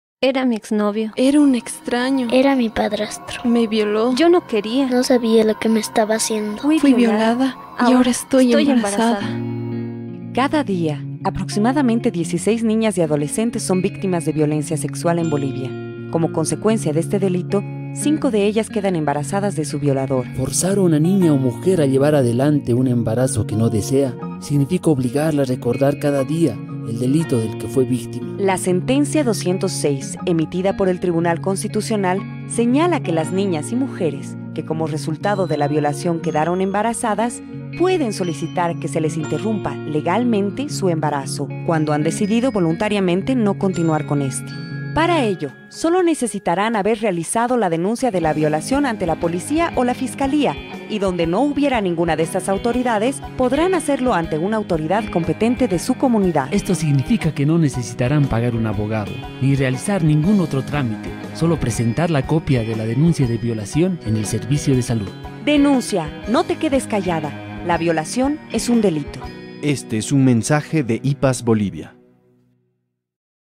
A SOLA DENUNCIA
En Bolivia, para interrumpir un embarazo no deseado causa de una violación o incesto se necesita presentar la copia de la denuncia al servicio de salud. La denuncia de la violación se hace ante la policía o fiscalía.

Esto es gracias a la Sentencia 206 emitida por el Tribunal Constitucional.